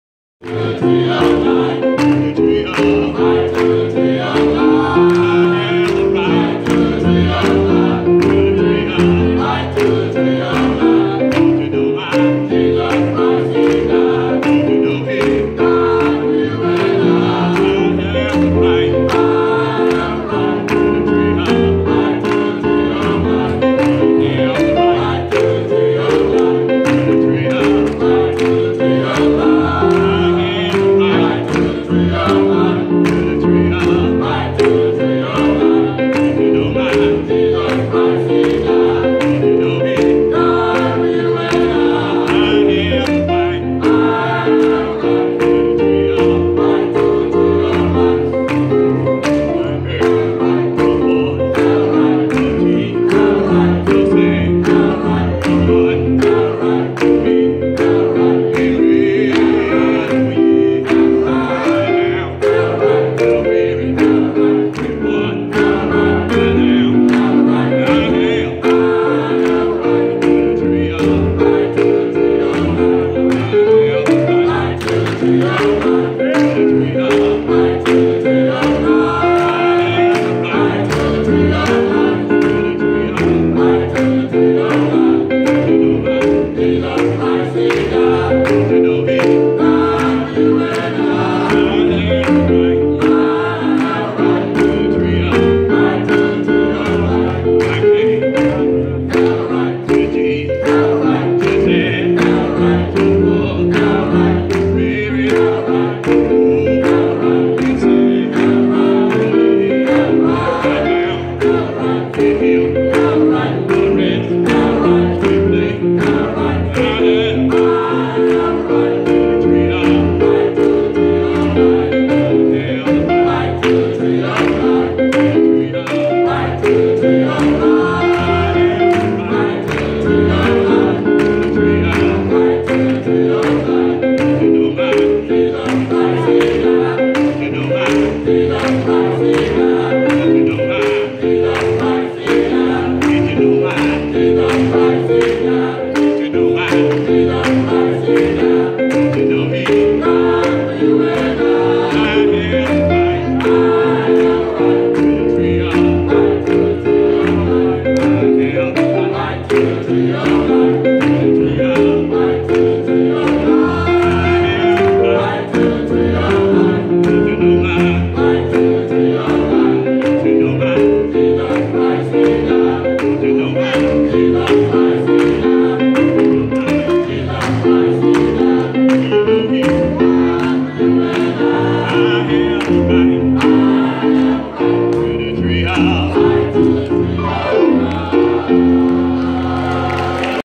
North MS Minister Association
At Pastor Waverly Sanders Anniversary Banquet